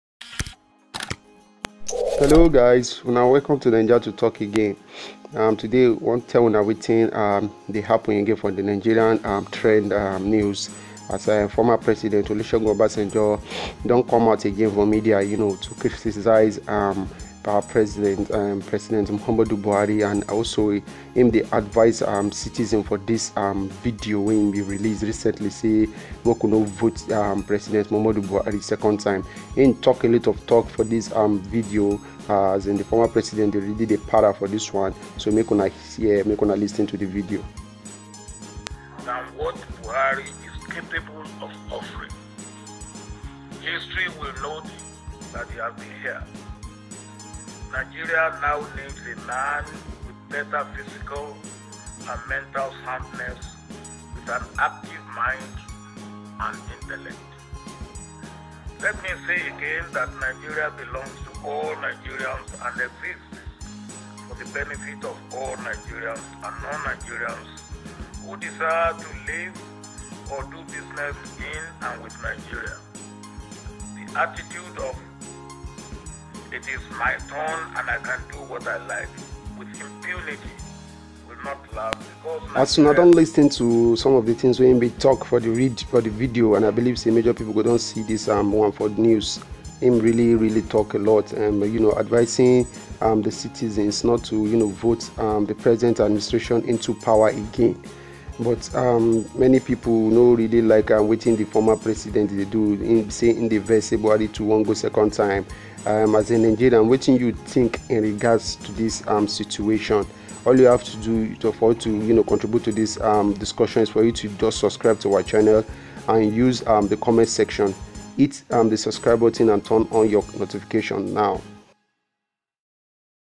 Obasanjo speaks against President Muhammad Buhari again
Obasanjo speaks against President Muhammad Buhari again, he appeal to Nigerians not to vote for the president in the upcoming General election.